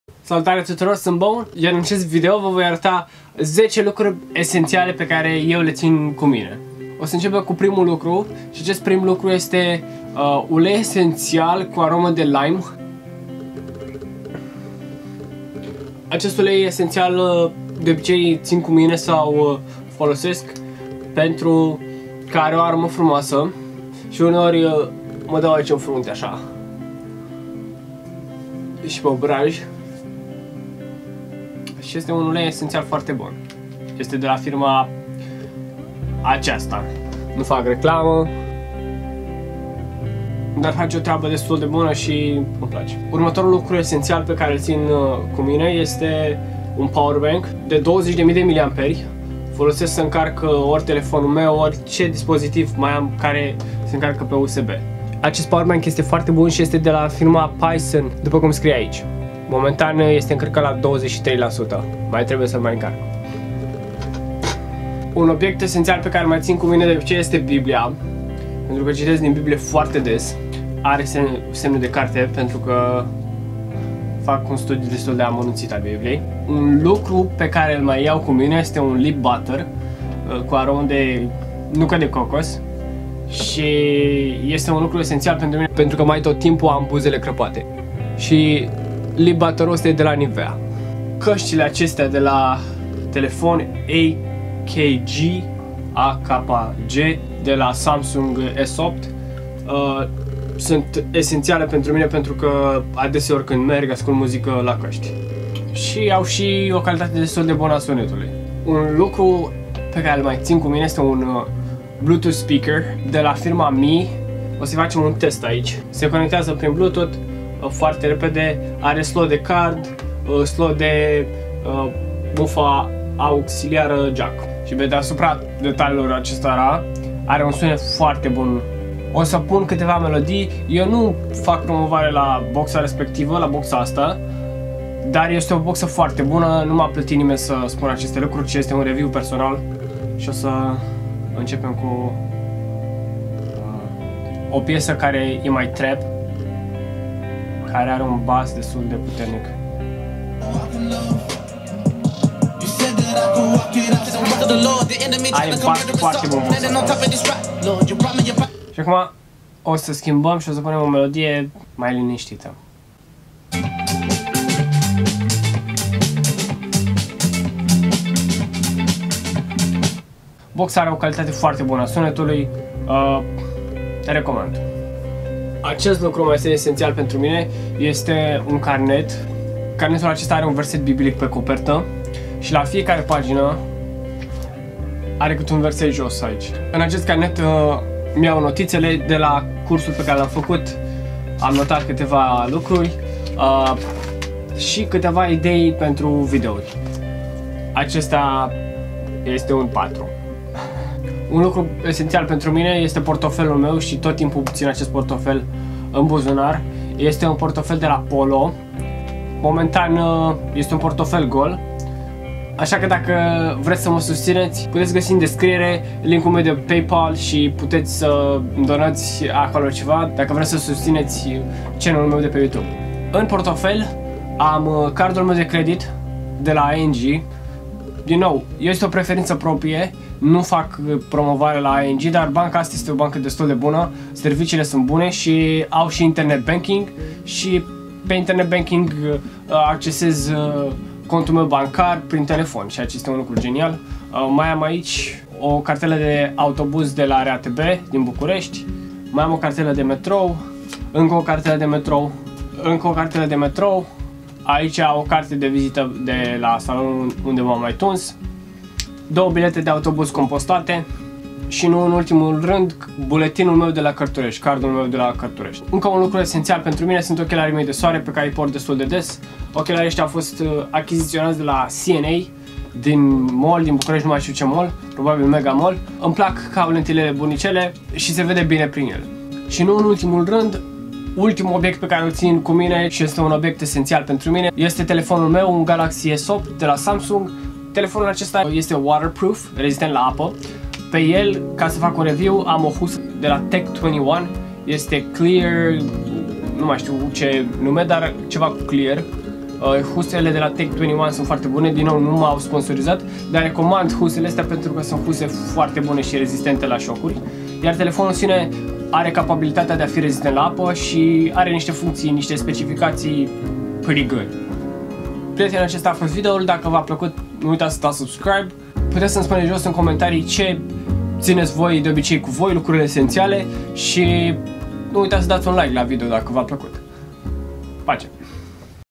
10 Lucruri esențiale pentru mine
In acest video prezint 10 obiecte esentiale din viata mea.